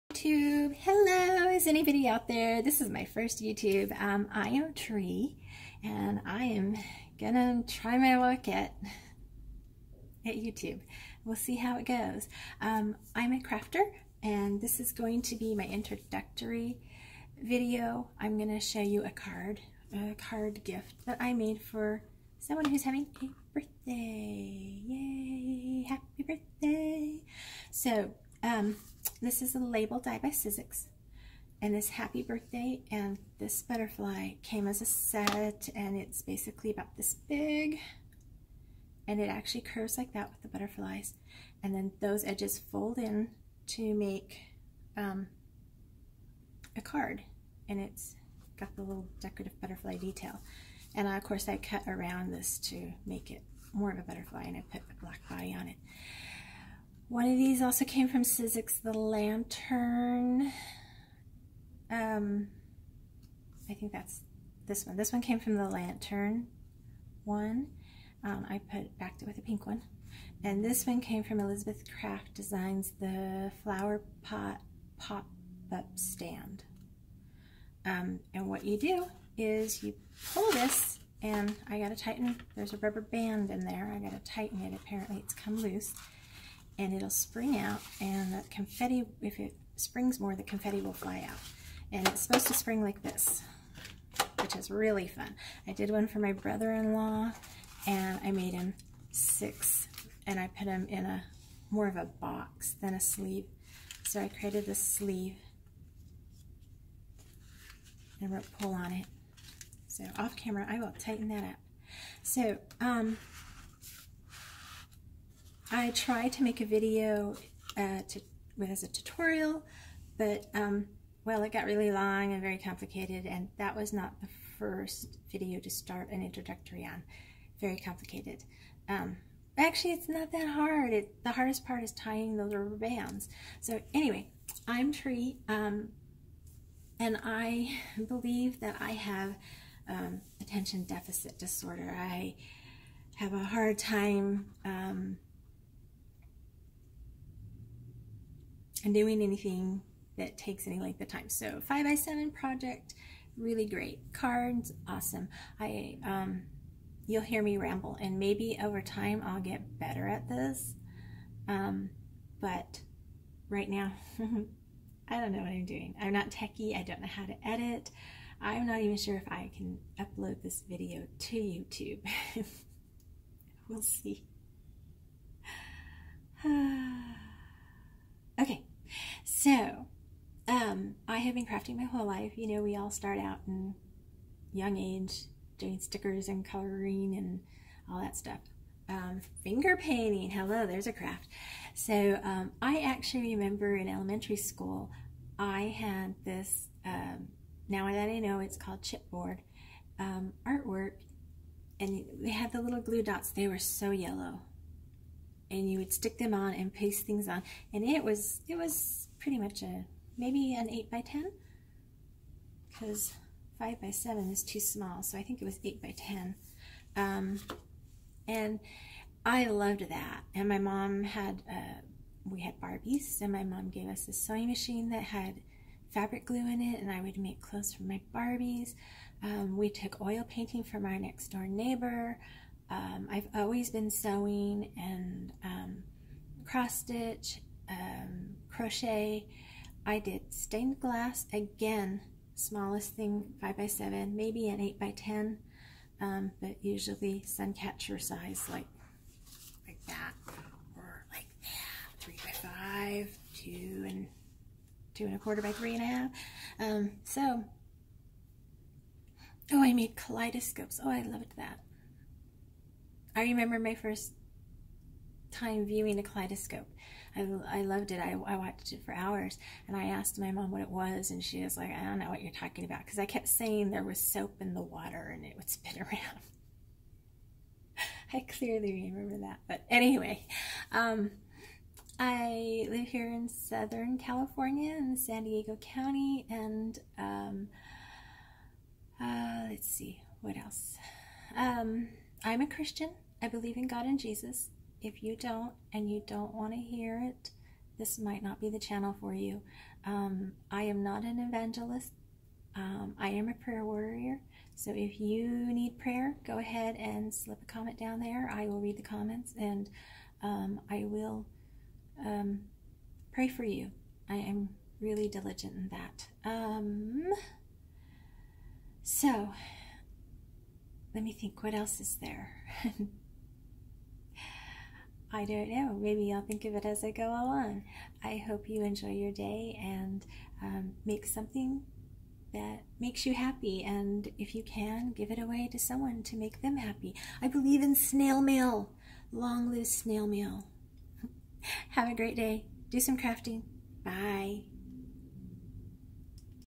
Hello! Let me Introduce Myself.
An introduction to who I am.  Walk-through of a birthday card I made.

Supplies used:
Park Lane 110# white cardstock
Authentique Latest & Greatest 6X6 cardstock pad
Scotch Tacky Glue
Rubberbands
EK Tools reverse tweezers